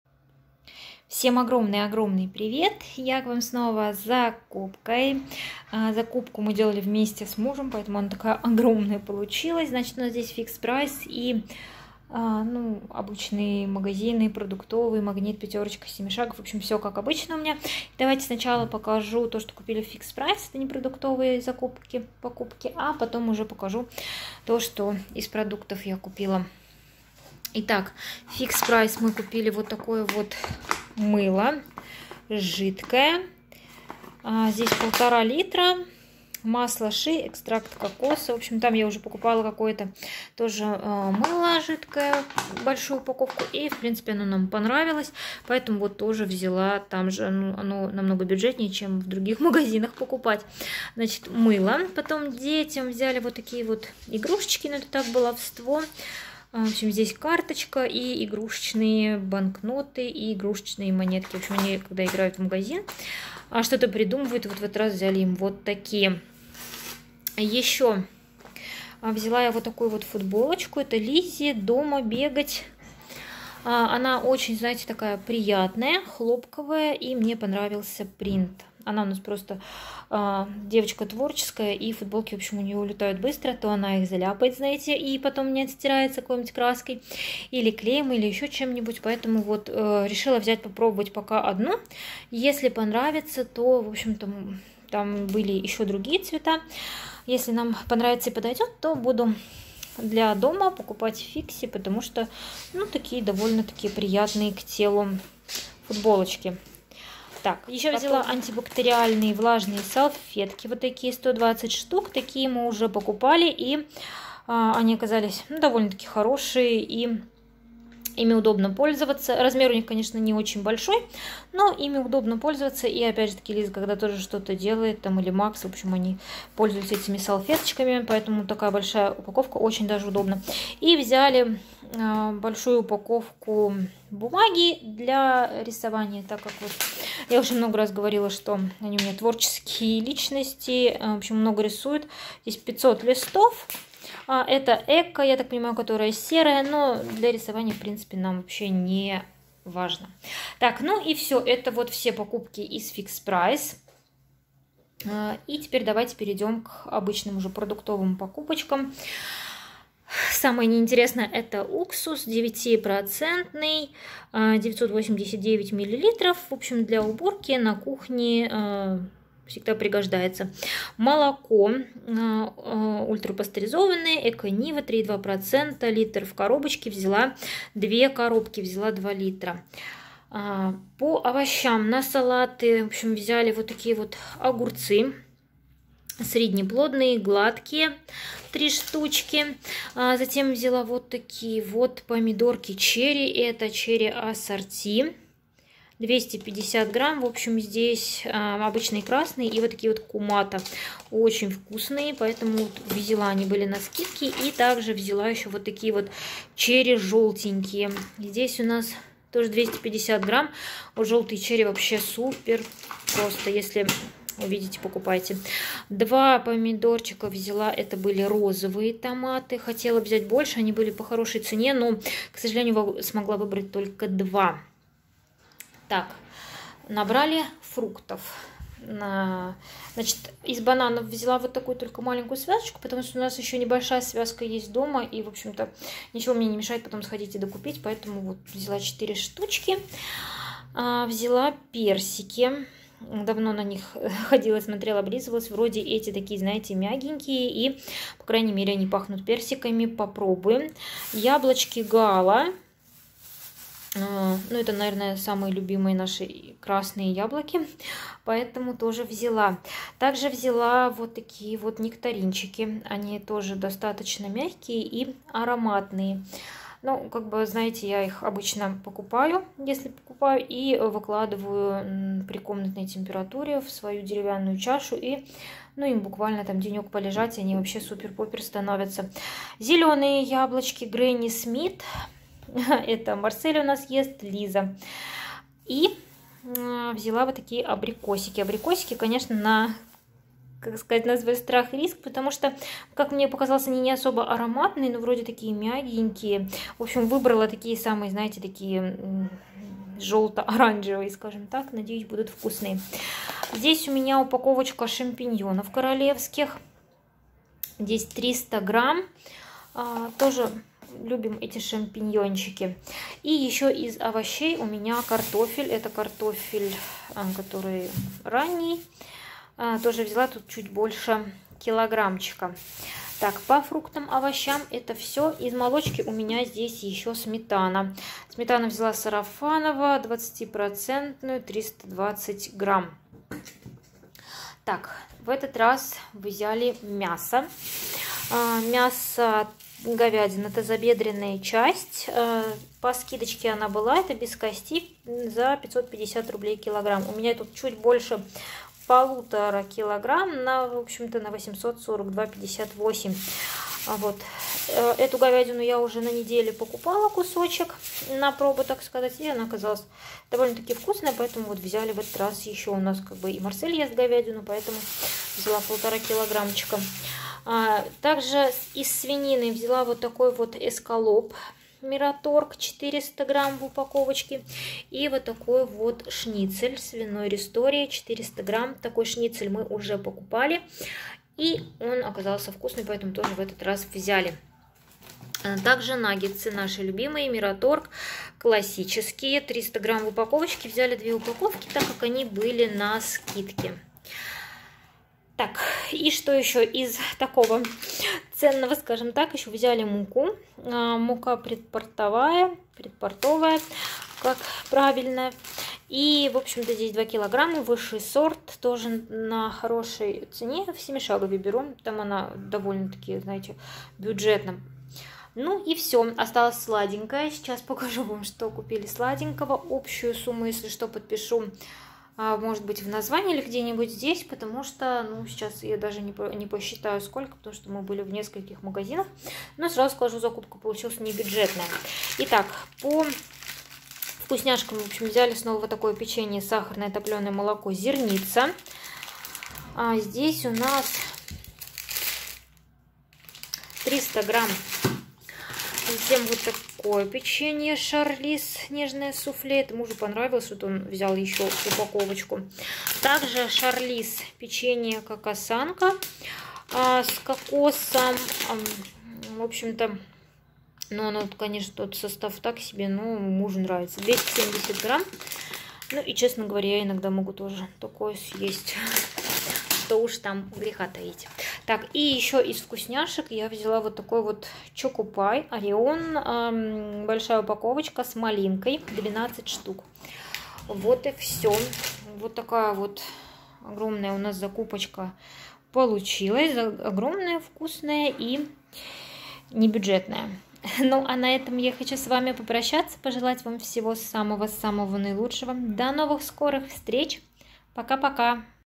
Огромная закупка продуктов с ценами на 4500 руб Июнь 2022 Санкт-Петербург
Всем привет, меня зовут Светлана. Я живу в Санкт-Петербурге с мужем и 2 детьми.